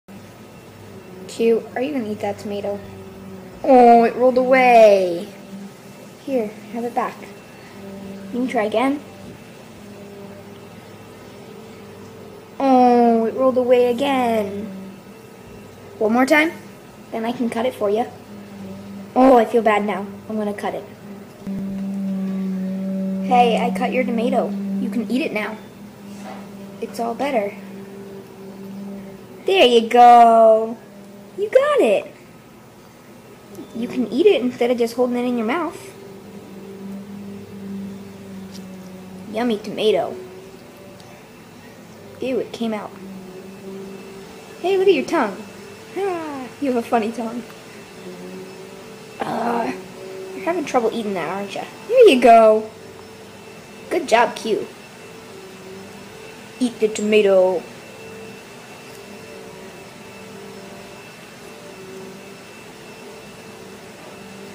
Turtle Trying to Eat a Cherry Tomato
my turtle likes tomatoes but I usually cut them for her. I was bored and got it on video.